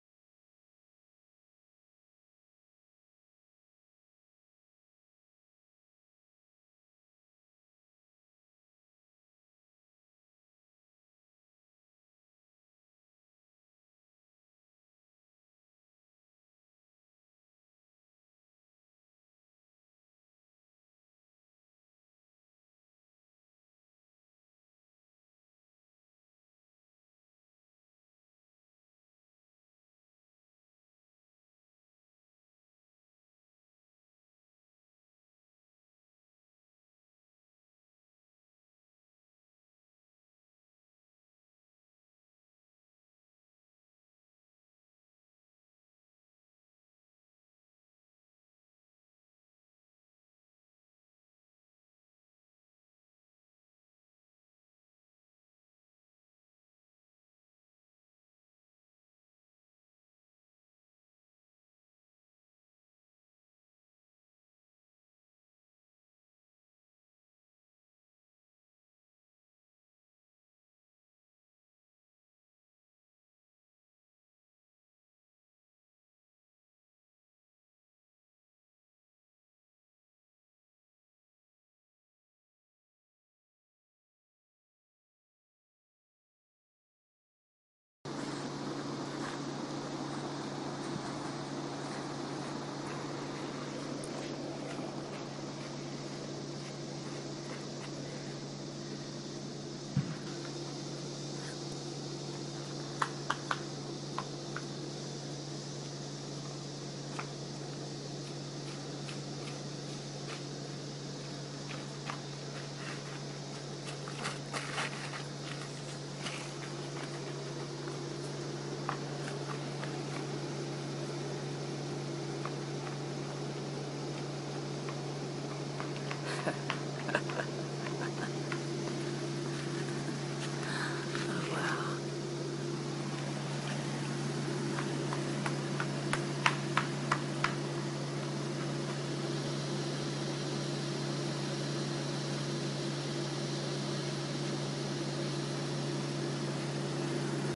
Chasing Butterflies With GoPro On Pole Mount! | MicBergsma
First half of the video filmed by gopro
Last half of the video filmed by Iphone4